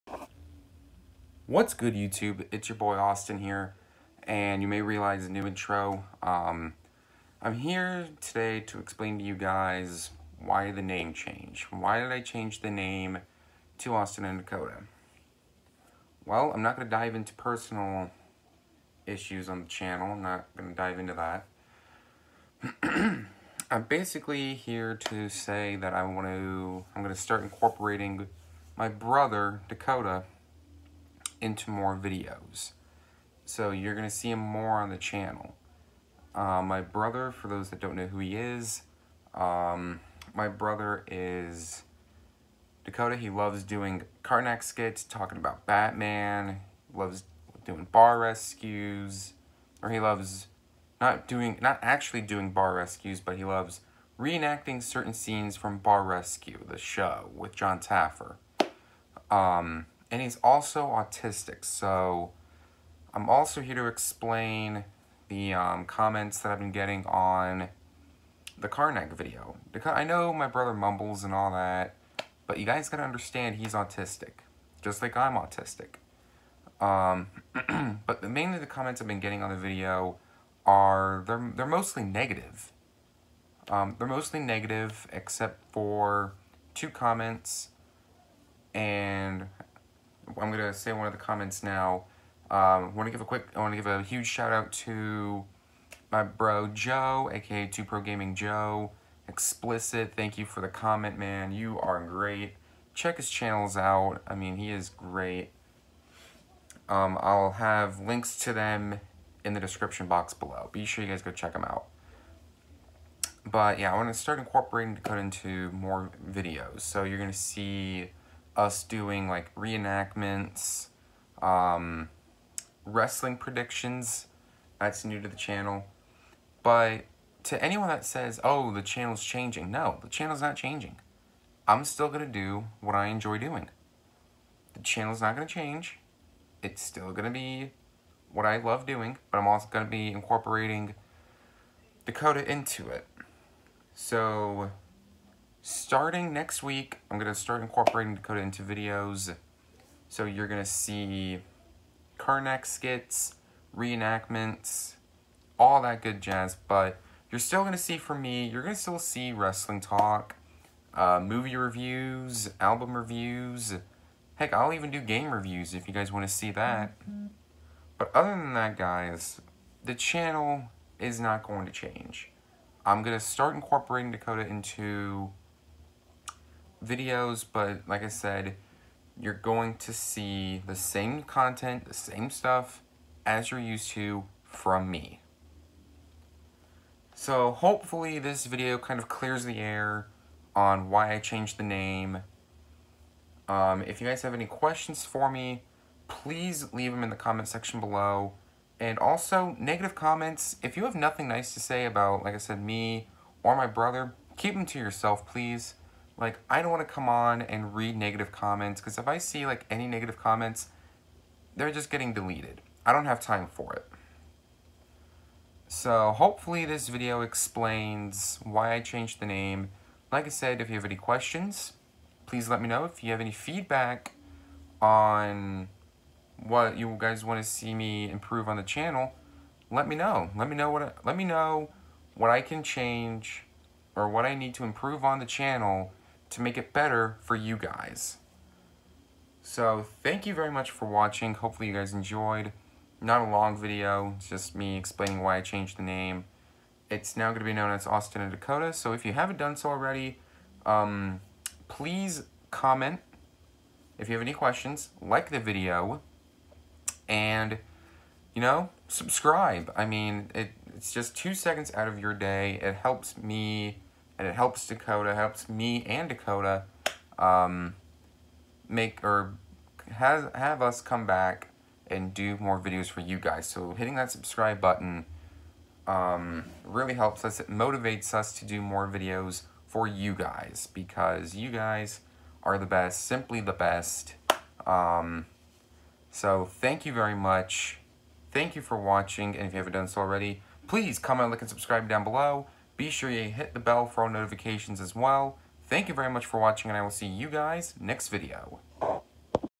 Answering Why I Changed the Channel Name
In this video, I answer why I changed the channel name.